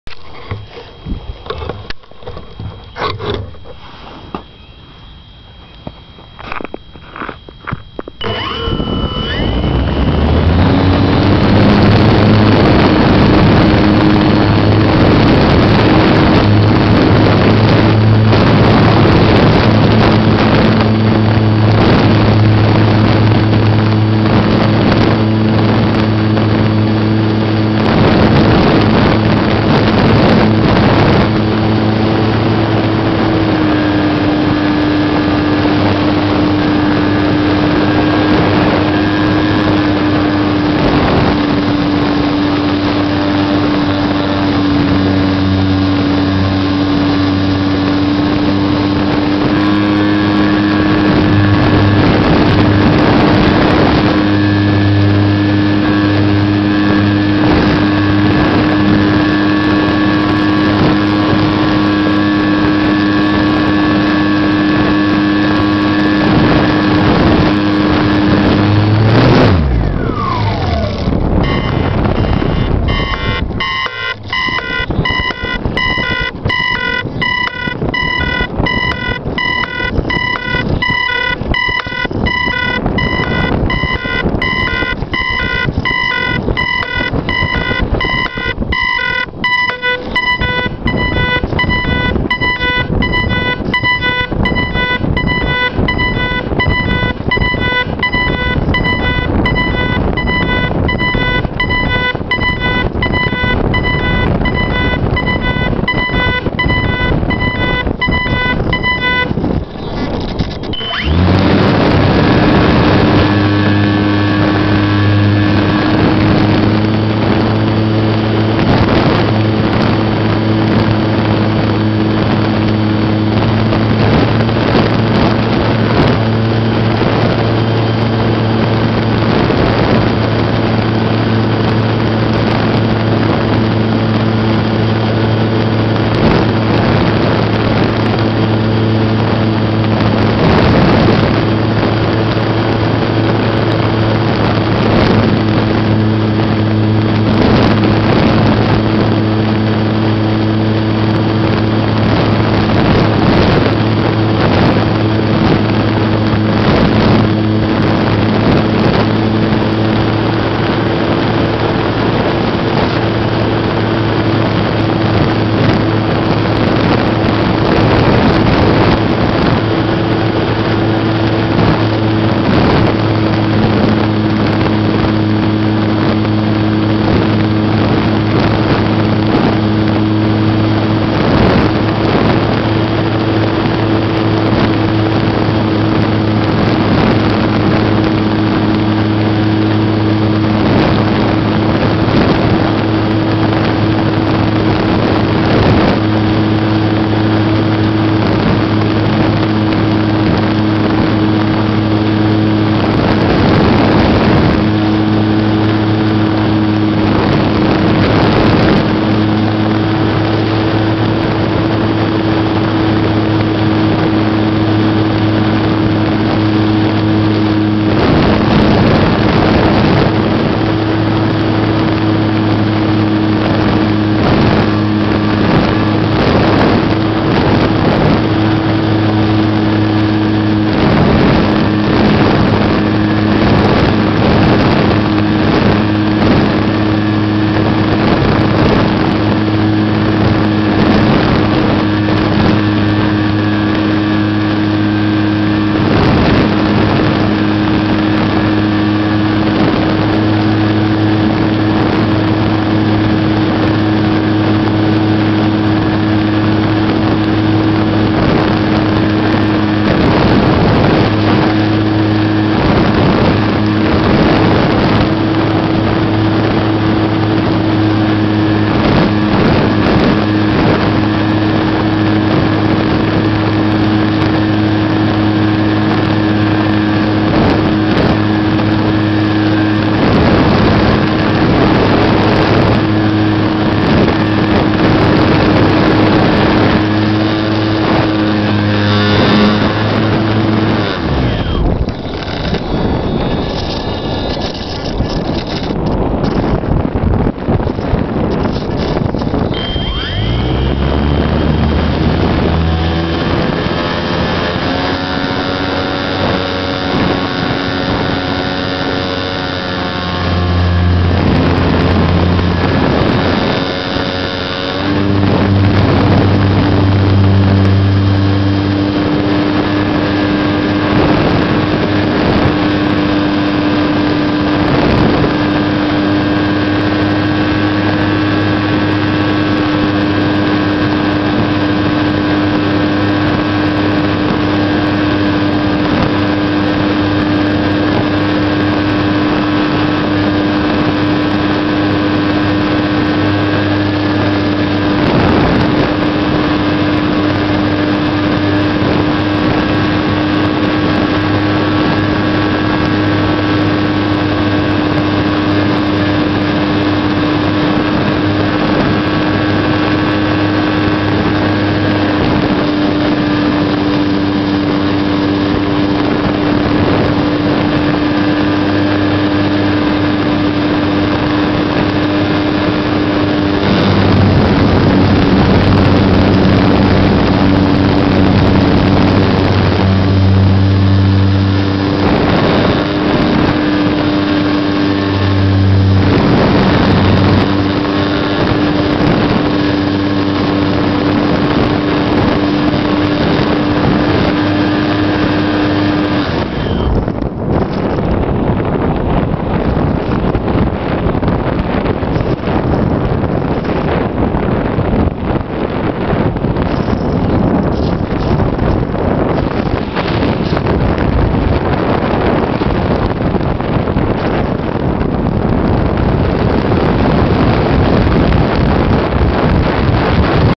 Flying my GWS Slow Stick over NE Beebe Arkansas after a good snow
Flying my GWS Slowstick around and high above my home in Beebe Arkansas after about a 6 inch snow on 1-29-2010.  It has mech. problems shortly after takeoff but just before I go for an emergency landing I figure out what's up and continue the flight.  The camera runs out of power about 30 sec. before landing.